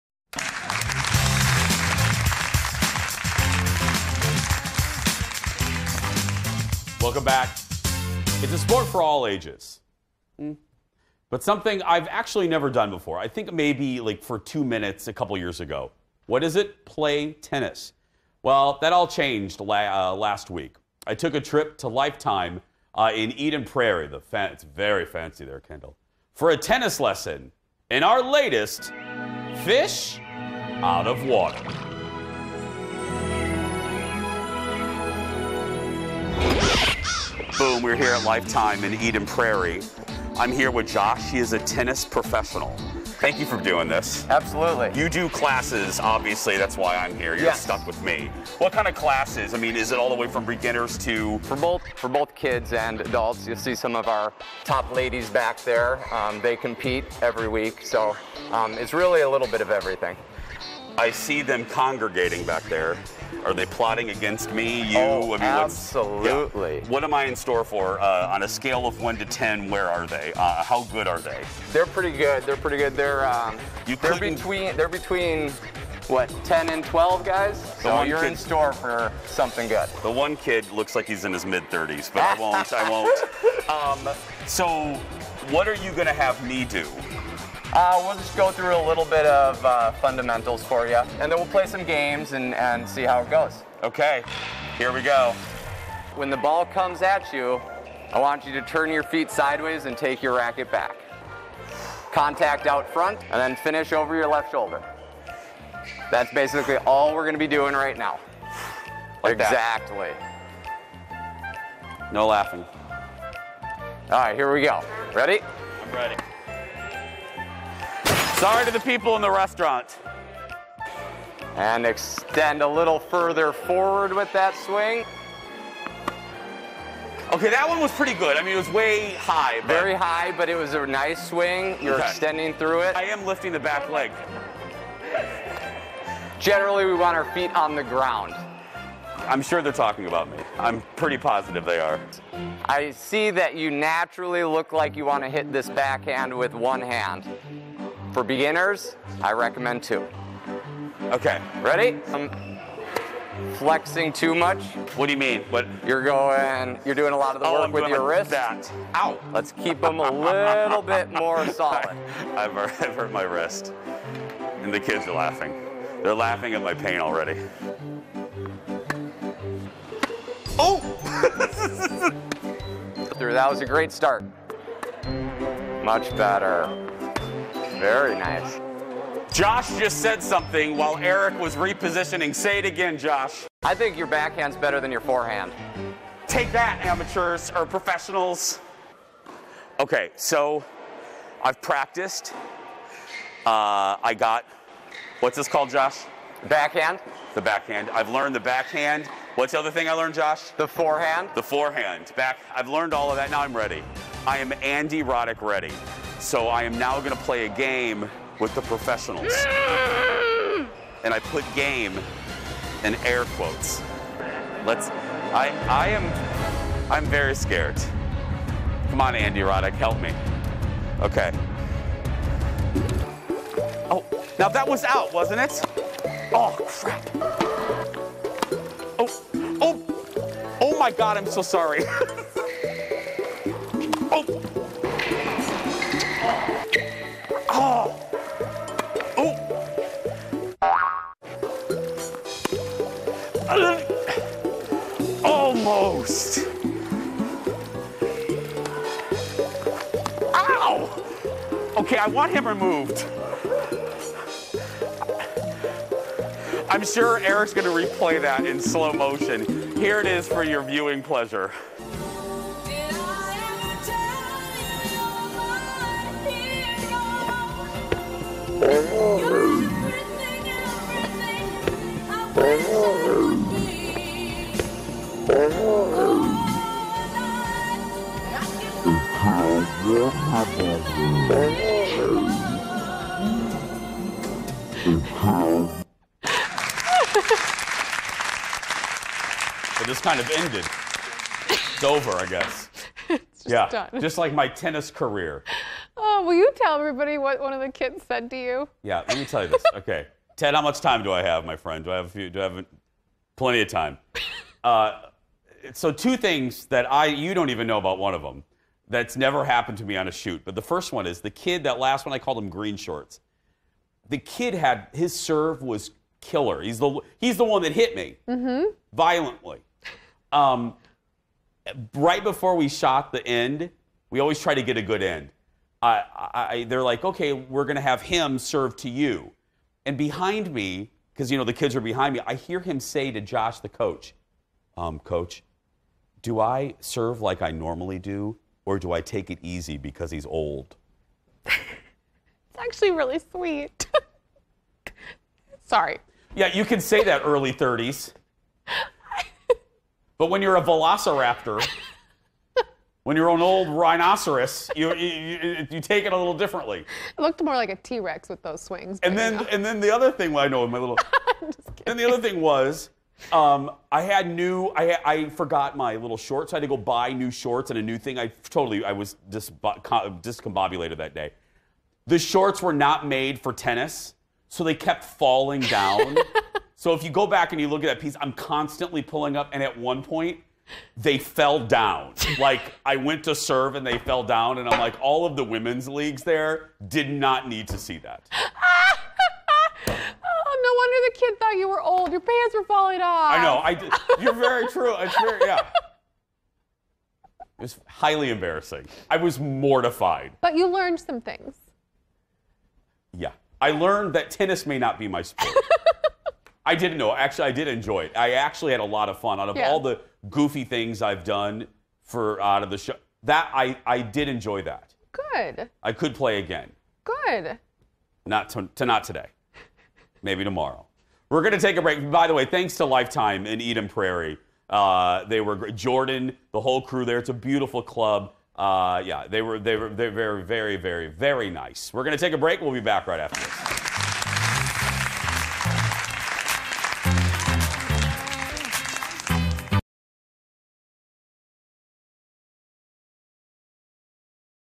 Jason learns how to play tennis with kids!
Jason has never played tennis before. He recently visited Lifetime to learn how to play with 10-12 year olds.